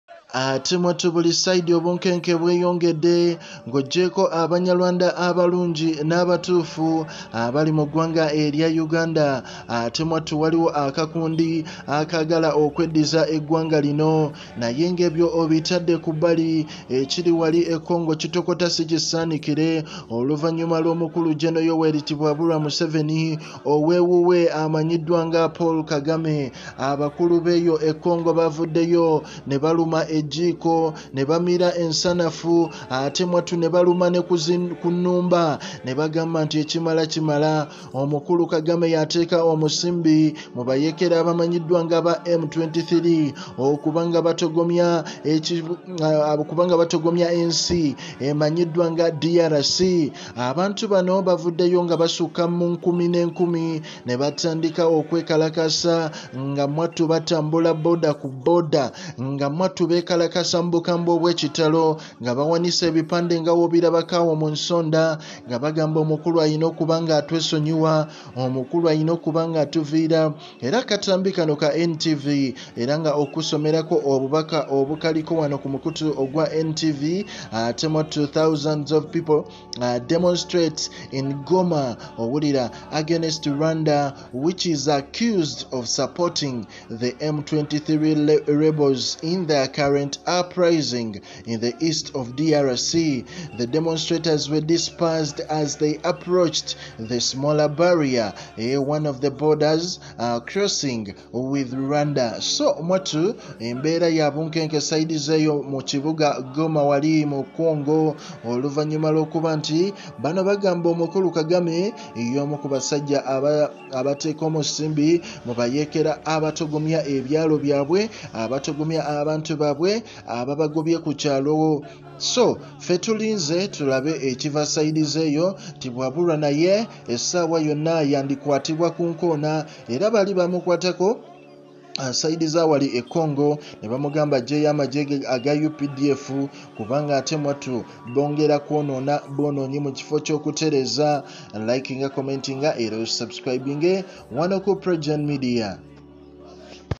luno lutalo sinzigu! Museveni takyikiriza Owuwe agobedwayo ECongo kagame aba congo balayide ofa naye
Thousands of people demonstrate, in Goma, against Rwanda, which is accused of supporting the M23 rebels in their current uprising in the east of DRC.
The demonstrators were dispersed as they approached the small barrier, one of the border crossings with Rwanda